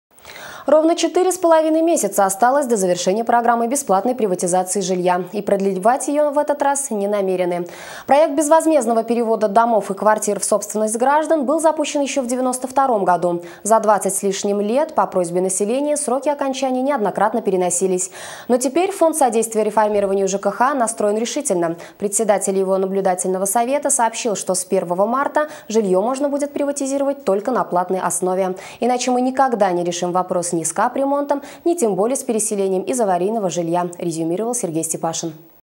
Успеть до весны
Ровно 4,5 месяца осталось до завершения программы бесплатной приватизации жилья. И продлевать ее в этот раз не намерены. Проект безвозмездного перевода домов и квартир в собственность граждан был запущен еще в 92 году.